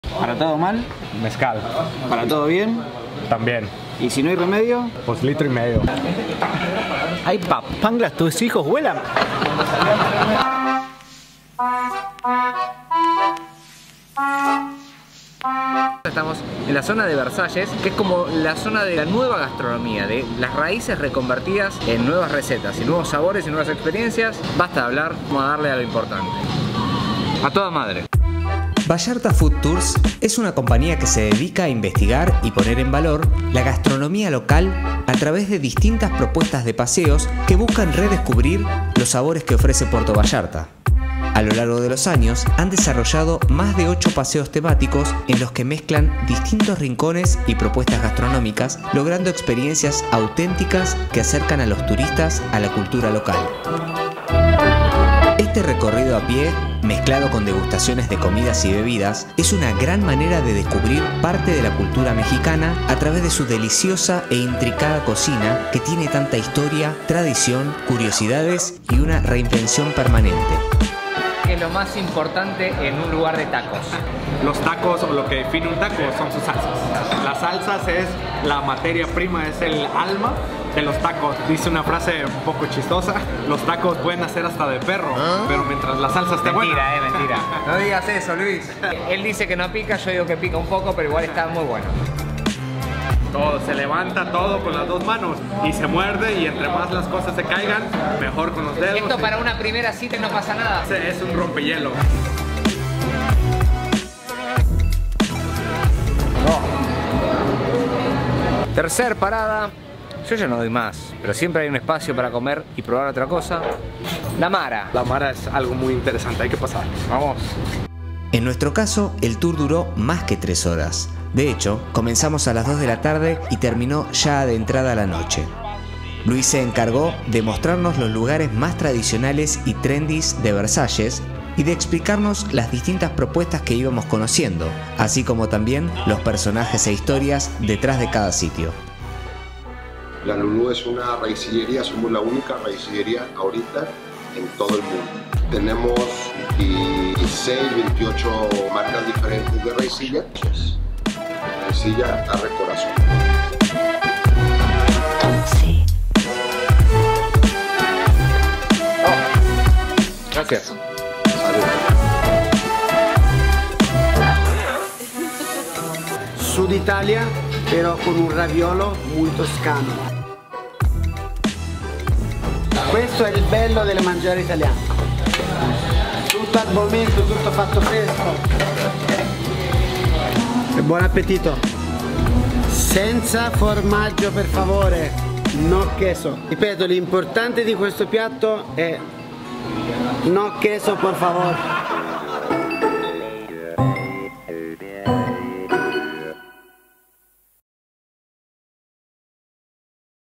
FOOD TOUR PuertoVallarta
Salimos en búsqueda de los mejores rincones de Colonia Versalles, uno de los barrios más cool de Puerto Vallarta. Allí, junto a Luis pasamos un día probando los sabores originales y nuevos de la gastronomía local.